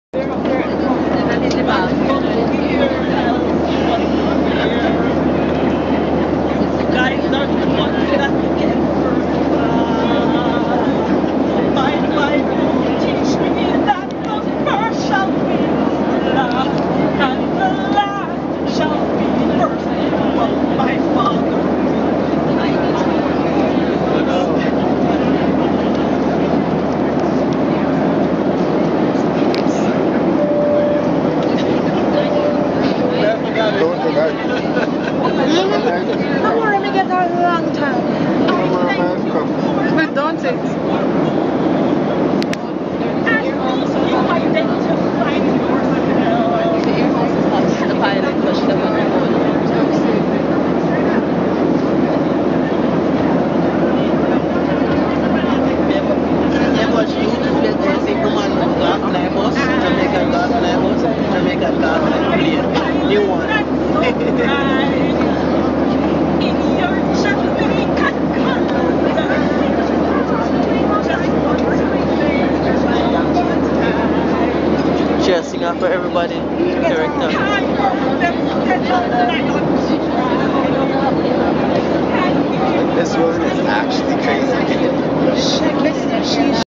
Mad woman threatening us with nail file- ALL RIGHTS RESERVED TO SWEETS LAWRENCE
@ this stage was staring on the passengers and singing a lot of crap and she had a nail file poking and threatening passengers on the flight!